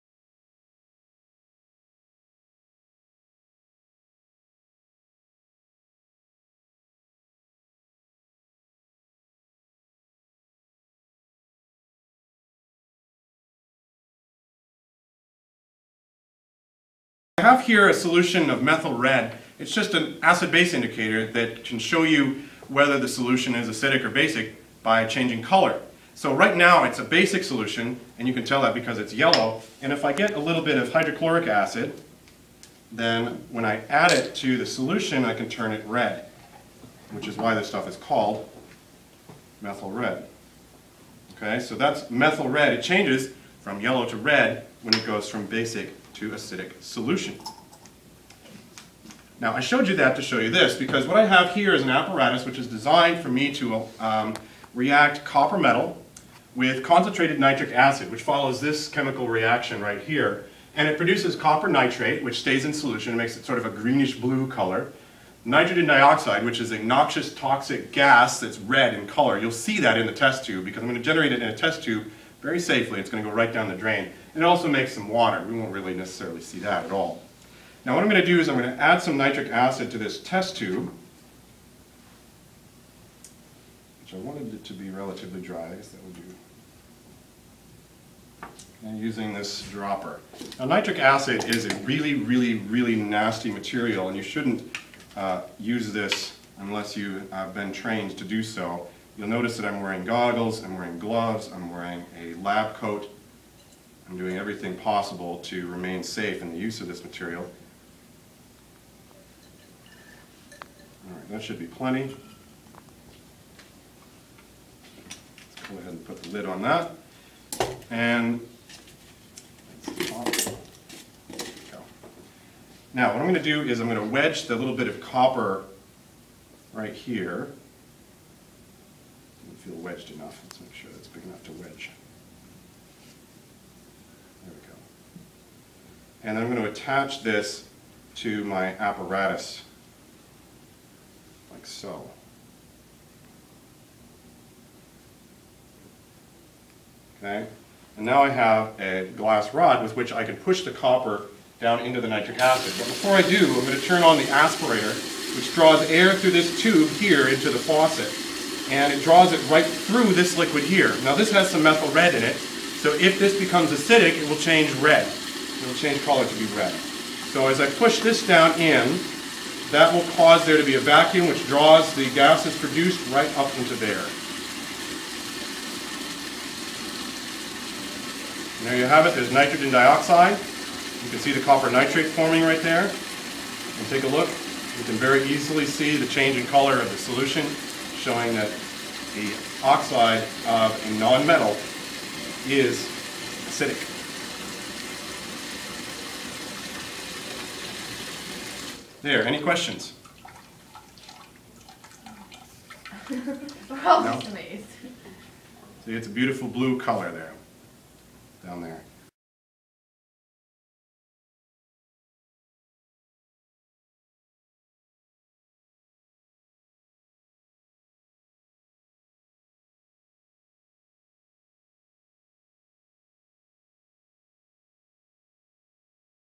Descriptive Chemistry: Copper and Nitric Acid
In this video Mr. Keller demonstrates the reaction between copper metal and concentrated nitric acid. The reaction produces nitrogen dioxide (a reddish-brown gas), copper nitrate, and water. By carrying out the reaction in a test tube connected to a flask containing an acid-base indicator and an aspirator the toxic nitrogen dioxide is drawn safely down the drain. When the gas passes through the acid-base indicator solution it causes it to change colors in a way that shows that the oxide of a non-metal, nitrogen, is acidic.